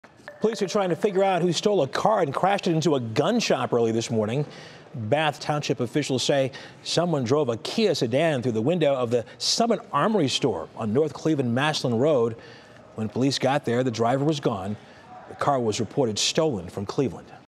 Stolen Kia from Cleveland crashes into Summit Armory in Bath Township
A vehicle stolen from Cleveland crashed into a gun store in Summit County on Thursday morning.

At 4:28 a.m., the Bath Police Department was called after a burglary alarm activation at Summit Armory on North Cleveland Massilon Road in Bath Township.

Upon arriving on the scene, officers from the Bath Police Department found a Kia that had crashed into the building.

Officers searched the interior of the building, but no suspects were found.

According to the Bath Police Department, the vehicle was reported stolen from Cleveland.